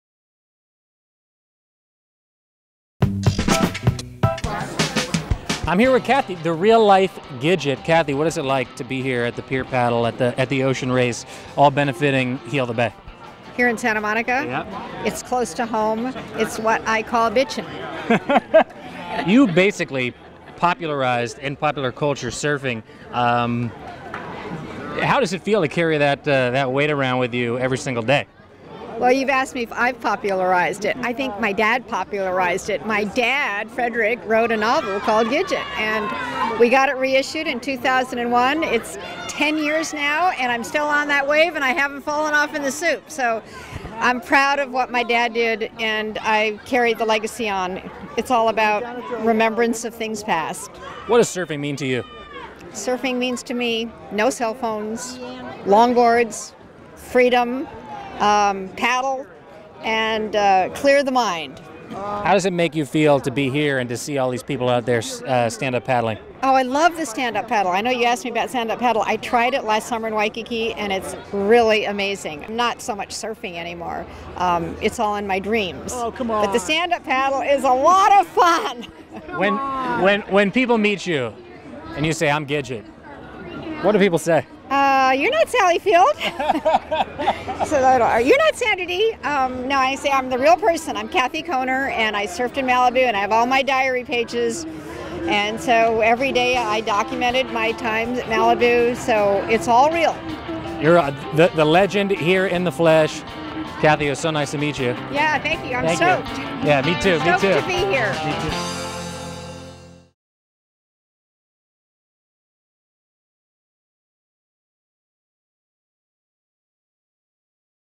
Interview: The Real 'Gidget,' Surfer Kathy Kohner Zuckerman
Sandra Dee may popularly be known as Gidget from the eponymous movie, but the character was based on Kathy Kohner Zuckerman, whose father wrote "Gidget: The Little Girl with Big Ideas" after he read her journal about surfing on the male-dominated beaches of Malibu. SoCal Wanderer host Jacob Soboroff caught up with Kohner at the Santa Monica Paddleboard Race and Ocean Festival in 2011 for a short chat.